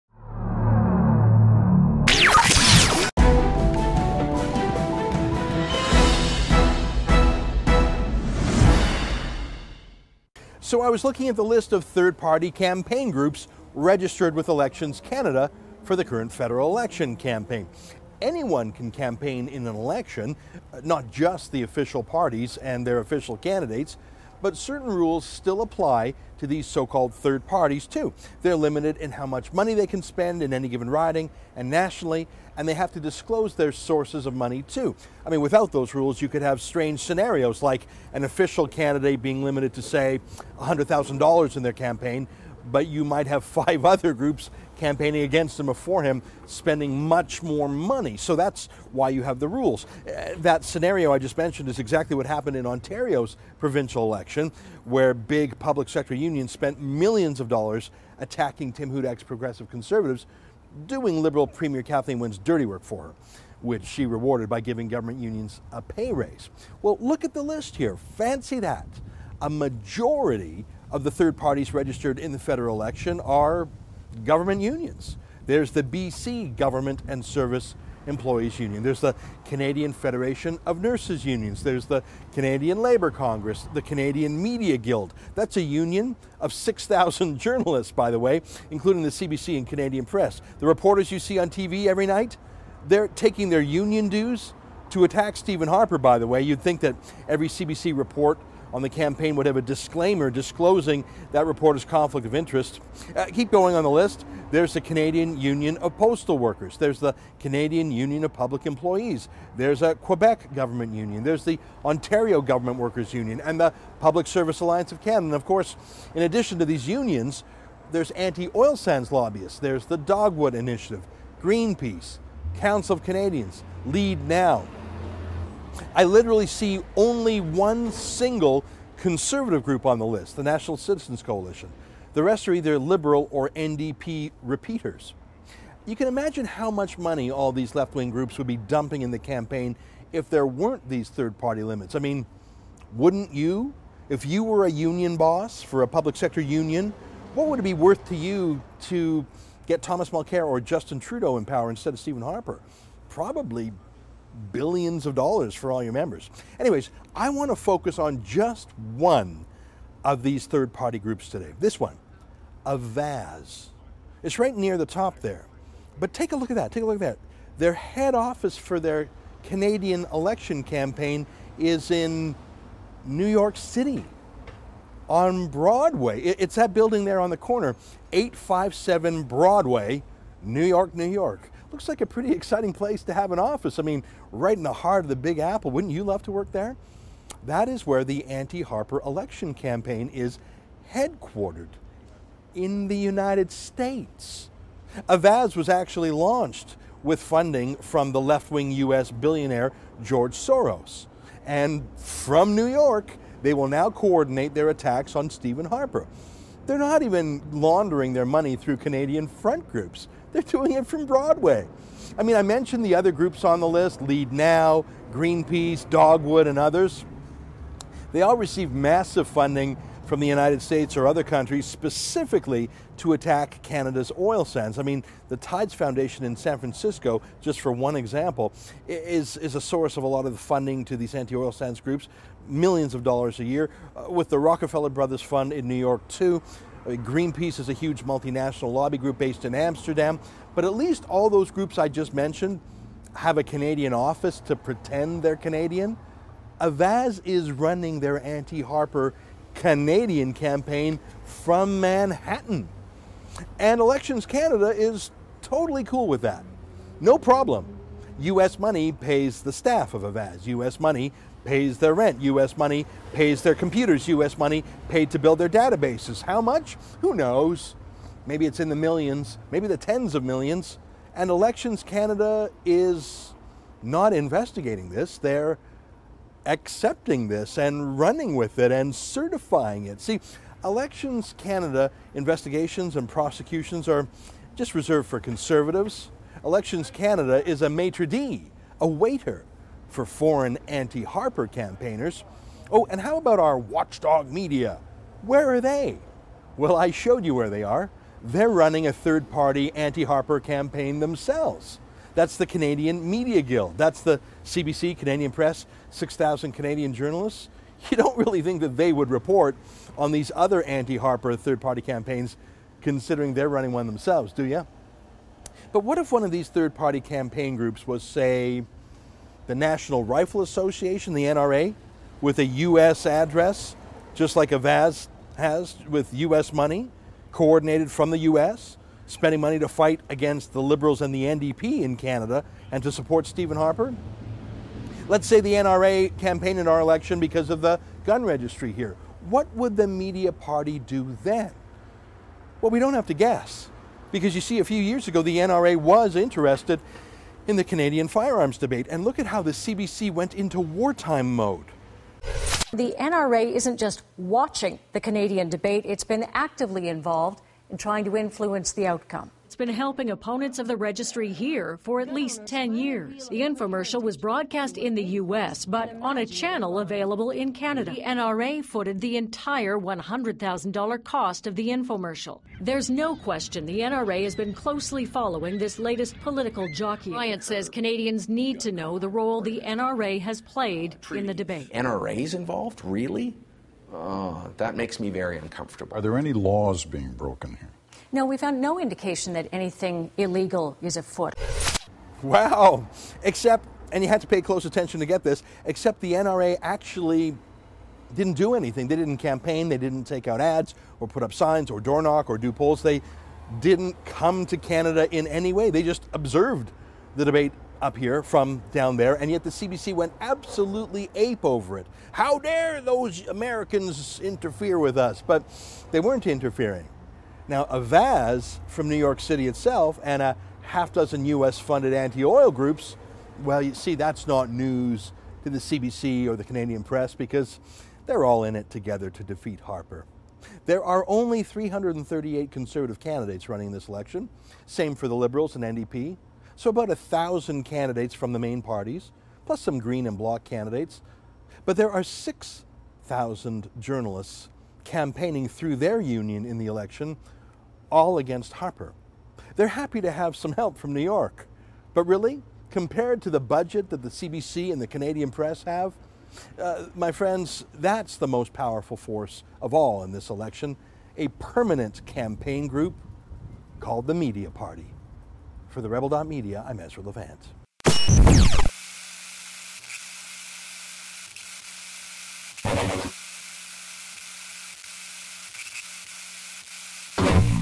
Elections Canada doesn't care about U.S. based Avaaaz
No surprise there: Most third party groups registered to campaign in the federal election are government unions, plus organizations like the Canadian Media Guild.

Ezra Levant of TheRebel.media reports that the list includes anti-oil sands lobbyists like Greenpeace. (There's only one conservative group on the list: The National Citizens Coalition.)

One of the anti-Harper groups is Avaaz -- whose head office is in New York City. Avaaz was launched by funding from billionaire George Soros. Lots of these third party organizations get money from the U.S., from Tides and the Rockefellers. But at least they have a Canadian office to pretend they're Canadian!

And Elections Canada has no problem with that. Neither does our "watchdog media."

Thomas Mulcair wants to be PM, but he holds dual citizenship.
SIGN THE PETITION telling Mulcair to choose: CanadaOrFrance.ca